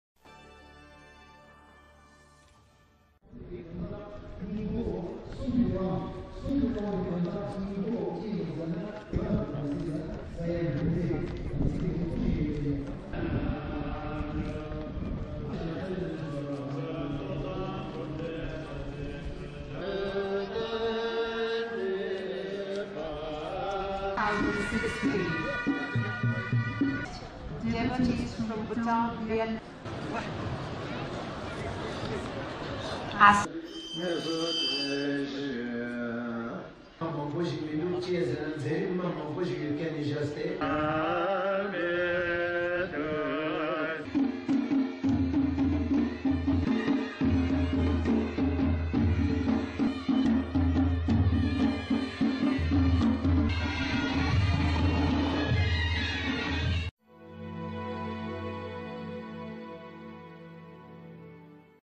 Naropa Festival-Kumbh Mela Of Himalayas Charges Air With Excitement
The air is charged with excitement in and around the famous Hemis monastery in Ladakh with gathering of devotees for the month-long Naropa festival in celebration of the millennial anniversary of the life and teachings of the eponymous Master of Tibetan Buddhism. It is being celebrated after a cycle of 12 years and is known as the Kumbh Mela of the Himalayas. The event this September, billed as Naropa 2016, falls in the Year of the Monkey on the Tibetan calendar. This year's Naropa Festival is the biggest showcase by the Drukpa Lineage of the Tibetan Buddhism headed by His Holiness Gyalwang Drukpa after 2004. He is the supremo of the great Drukpa Kagyu Lineage. Since Druk means dragon, it is also known as the Dragon Lineage.